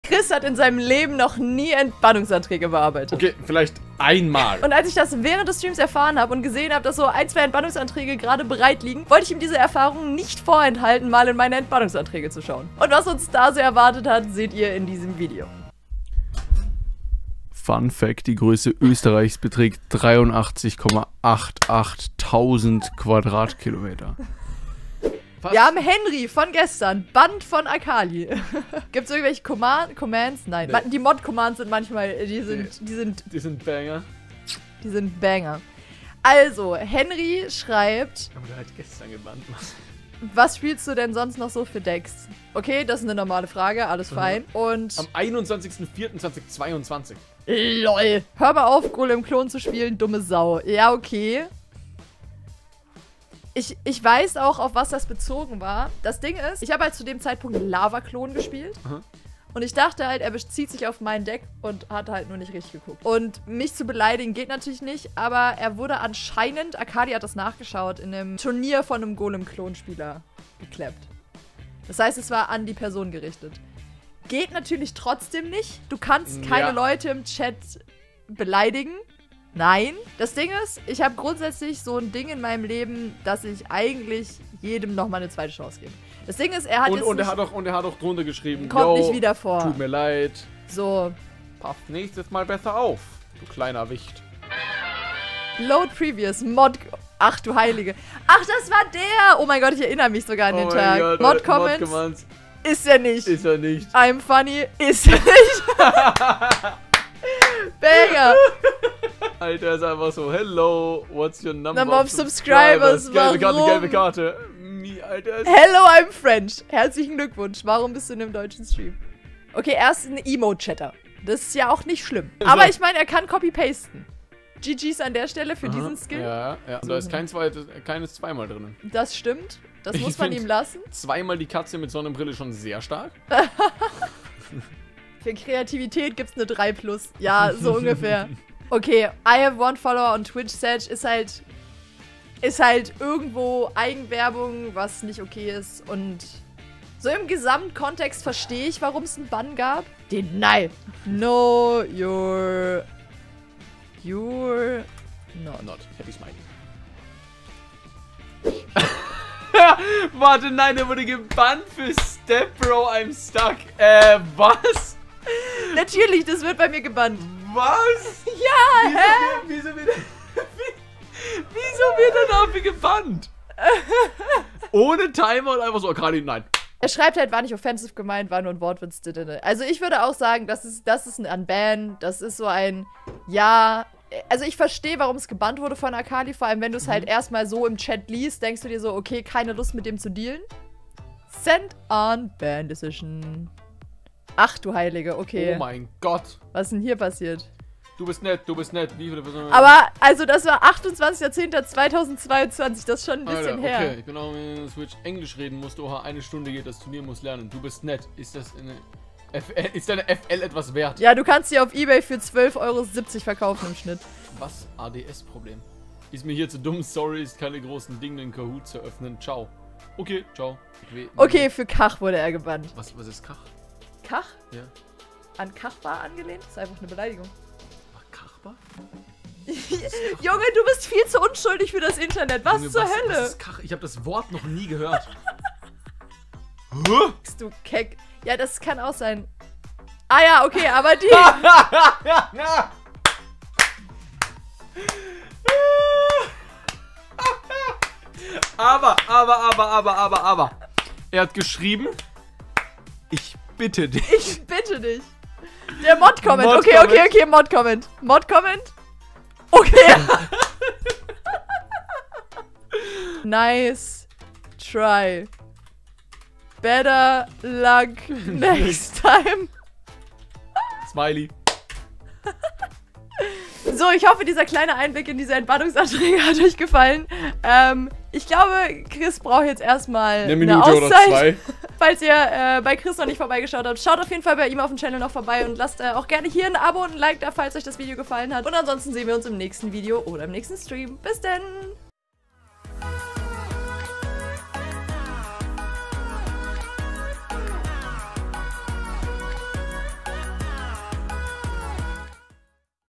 Entbannungsanträge die ZU WEIT gehen! mit
Entbannunganträge sind einfach immer wieder Comedy für sich. Chris war auf jedenfall nachhaltig geschockt. Wir sehen uns im nächsten Video oder im Stream!
Bis dann. ❤
Hier gehts zum ersten Teil: @jens.christensen